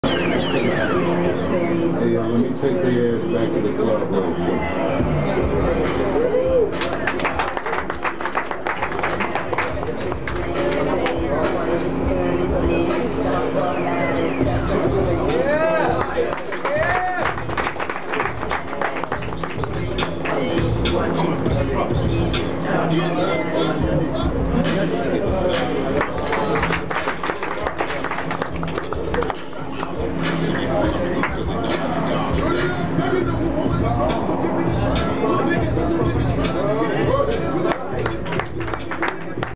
Full Planche, handstand push up, straddle planche push ups & impossible dip (tired)
Was a big and long day at the brighton calisthenics event, this was my last set after all the comps and training with people, was a good set but flopped the impossible ;) still continued and managed to do a messy one at the end